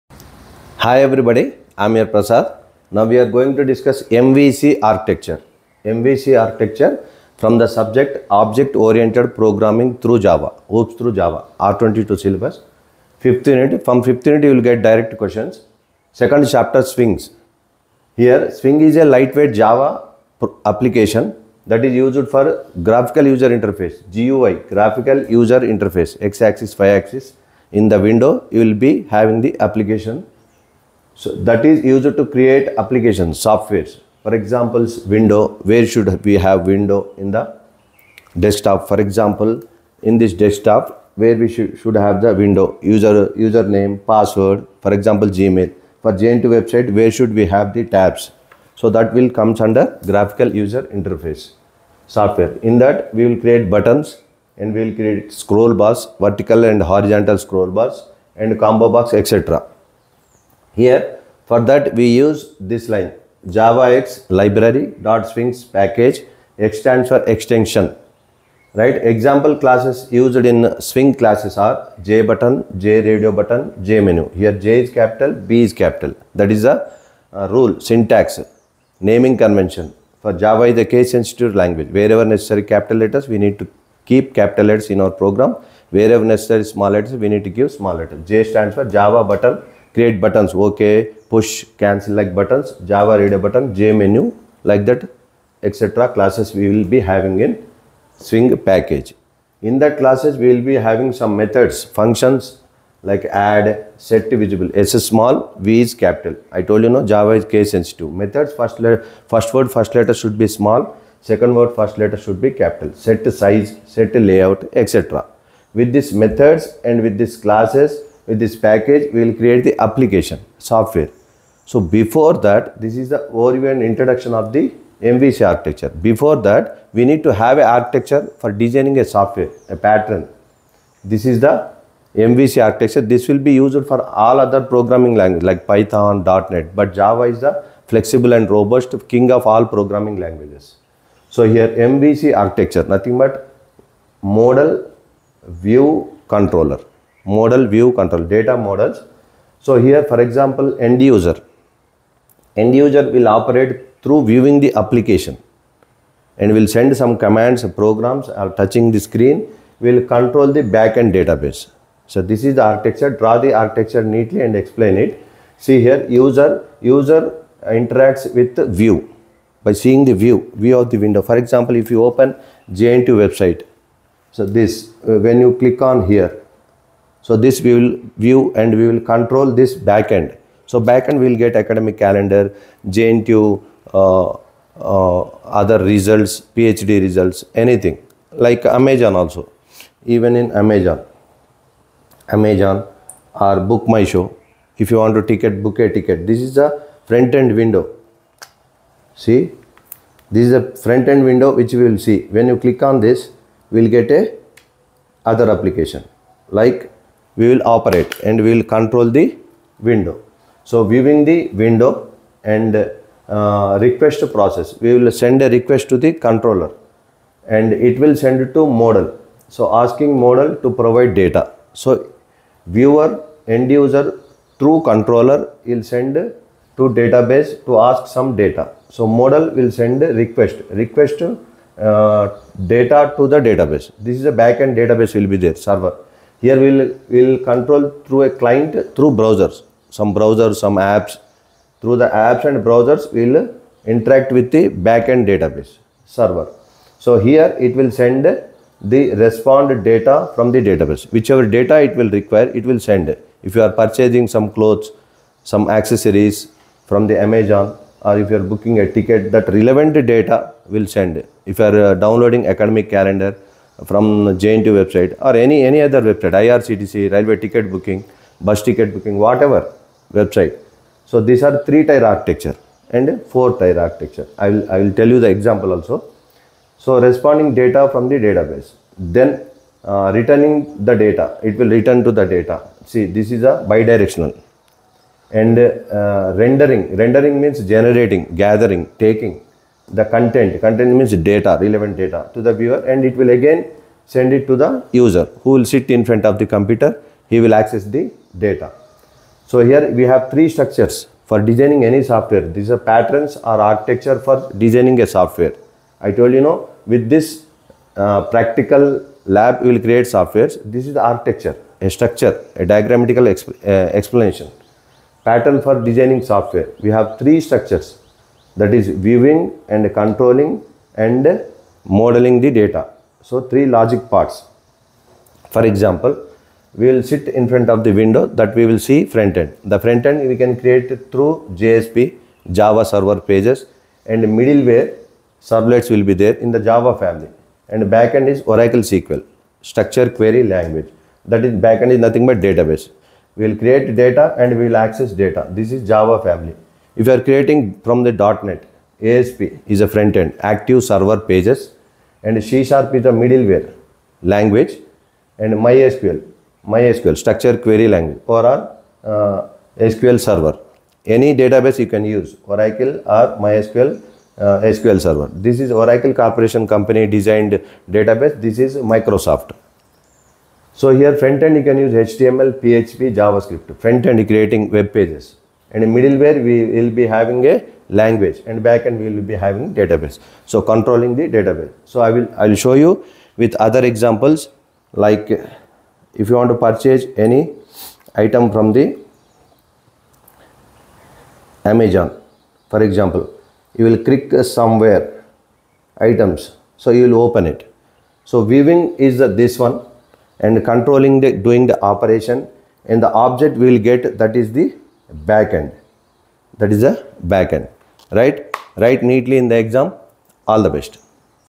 MVC Architecture / MVCArchitecture / Swing/JAVA/PrasadSir/R22/JNTUH/ObjectOrientedProgramming
Here in this Video, I Explained about "MVC Architecture", Which is a Topic from 5-UNIT:- Swing.

00:00 Swing-Introduction
01:00 javax.swing-Package's-Classes
01:40 Methods in Swing
02:11 MVC-Architecture
02:51 Some Real World Example
05:28 Other Scenario
06:24 a Case Study

*JAVA; PyThon &  Seminar, Mini & Main Projects Information:-*